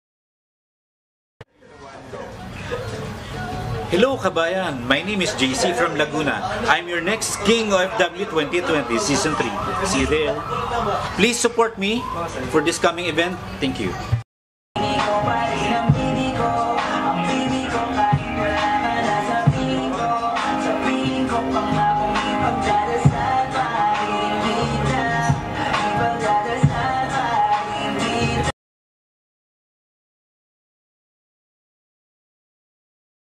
OFW KUWAIT KING & QUEEN 2020 Season 3
KING JC of LAGUNA!
One of the Auditionee for OFW KING & QUEEN 2020 s3 (2nd Audition)

FTS Production International Incorporated and Pnoy Talents Q8 is a non-profit organization that enhances, develop, innovate young Talents in Culture and Arts.